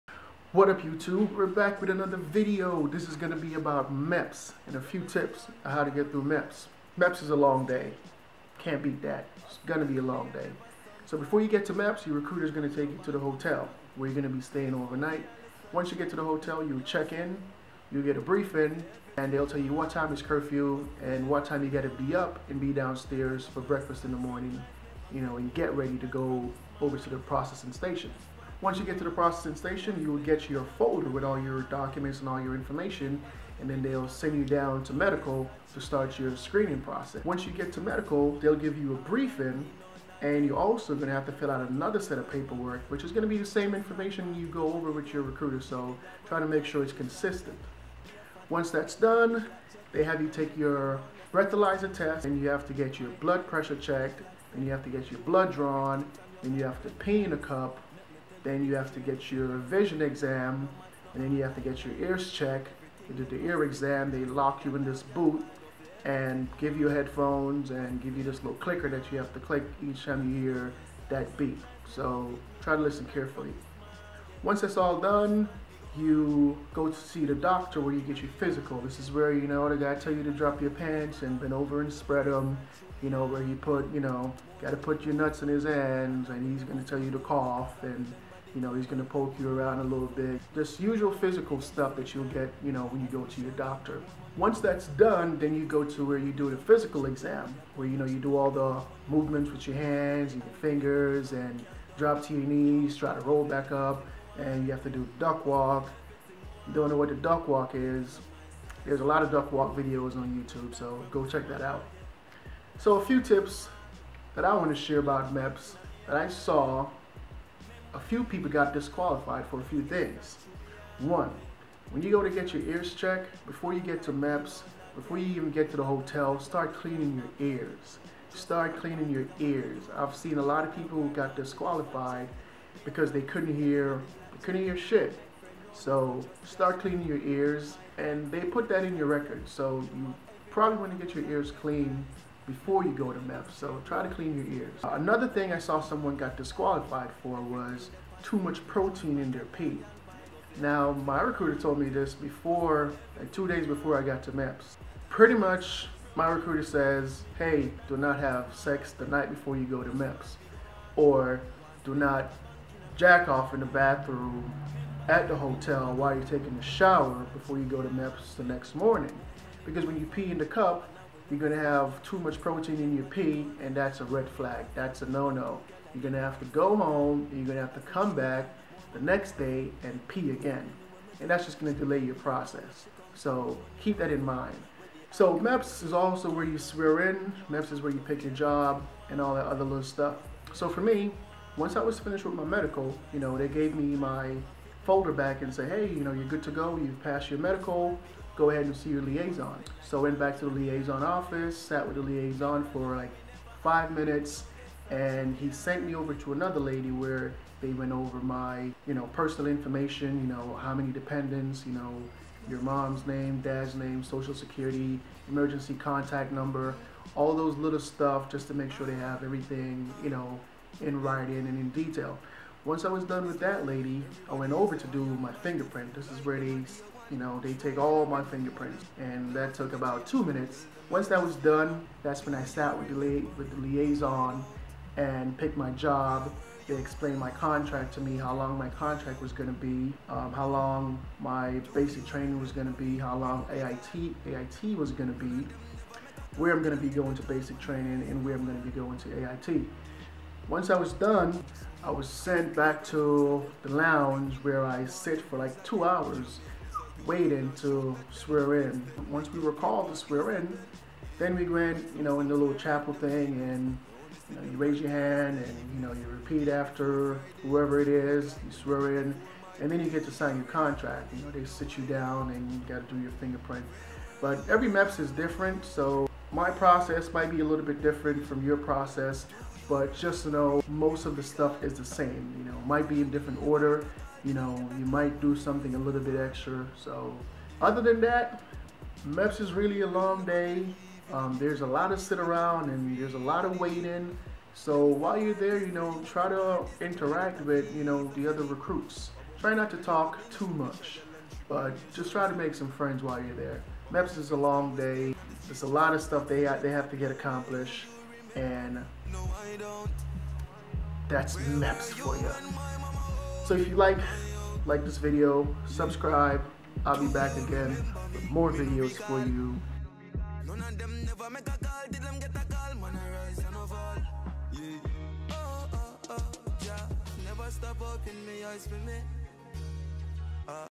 Joining The Army | MEPS Tips and Disqualifications 2016
My MEPS Experience | Dallas TX